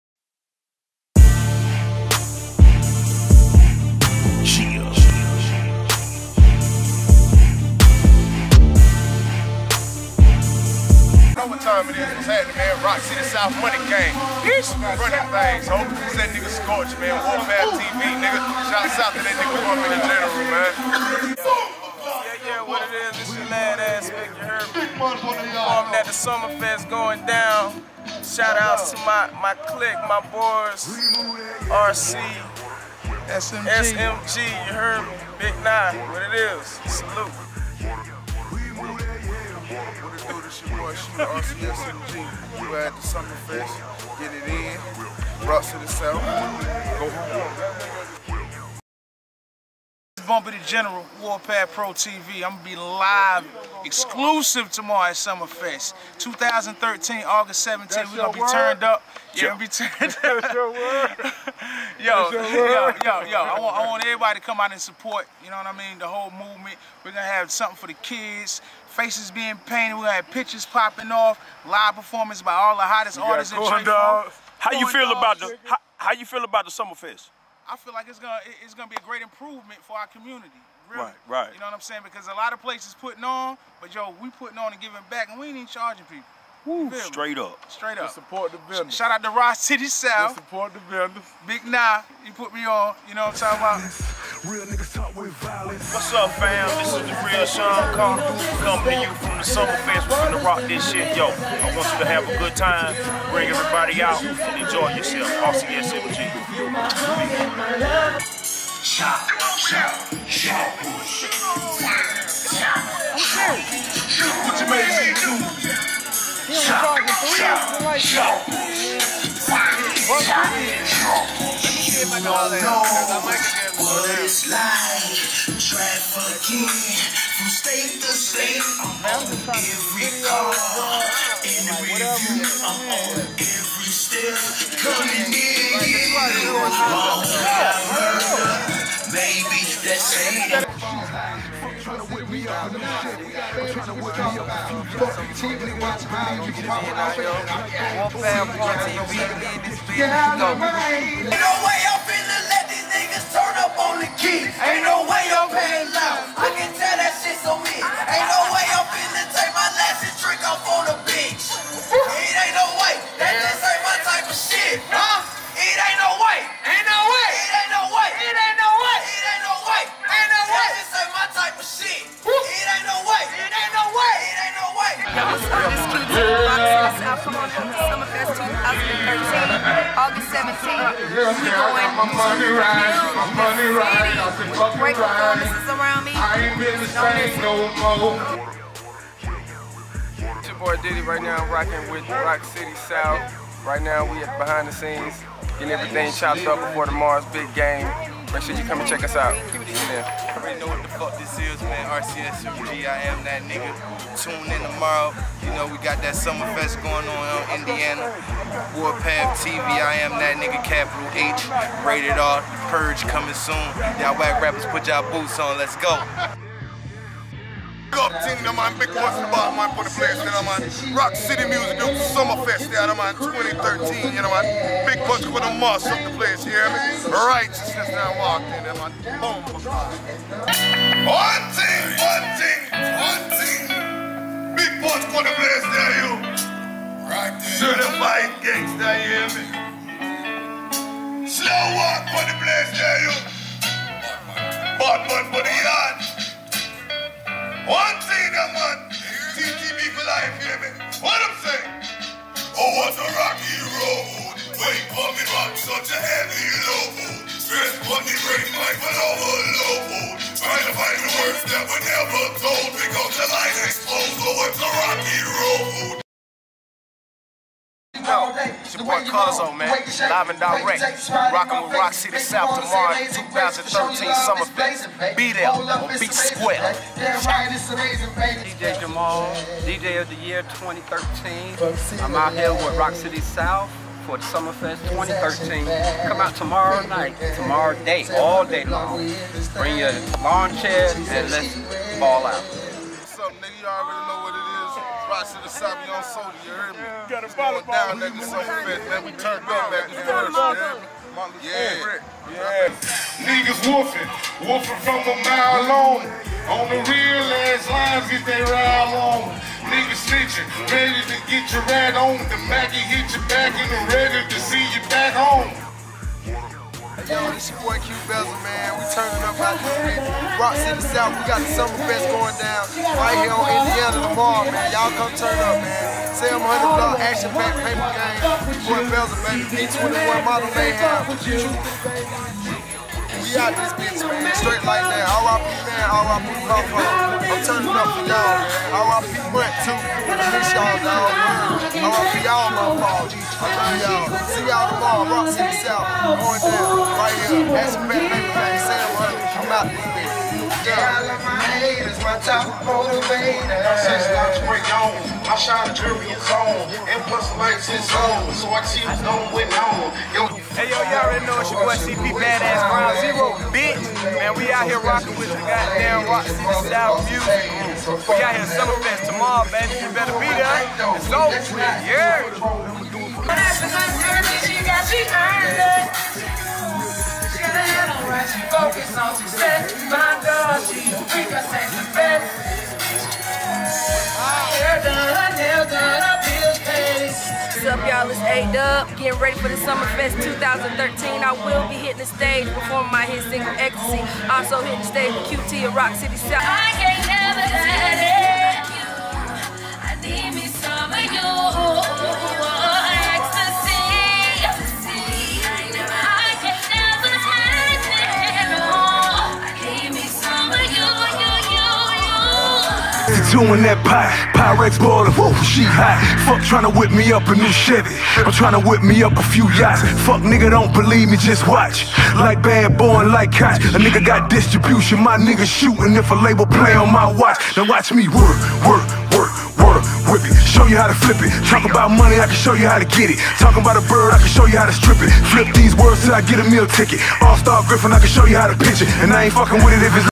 R.C.S.M.G-Summer FesT -REHERSAL - PART 2..FILM BY: WAR PATH PRO TV
WE will be Rocking The City # TODAY. LIVE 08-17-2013
 For the beginning of the new school year..
Plenty of food, Fun an Live Entertainment.
Comedy, Dance Contest,Hip Hop, R&B an much more..
This Is a Event for THE ENTIRE CITY.
WE GOT A BOUNCY HOUSE, FACE PAINTING,
SEVERAL LOCAL-MAJOR ARTIST COMING OUT SO BE THERE!
This a event you don't wanna Miss...
LFB AUTO&TOW-CLIFF LEAK, ZONE DAY CARE, THE WAY OF LIFE NON PROFIT PROGRAM..
ADD IT'S ALL HOSTED BY: BOBBIE BUGATTI
3912 INDIANA AVE W-S, NC- 27127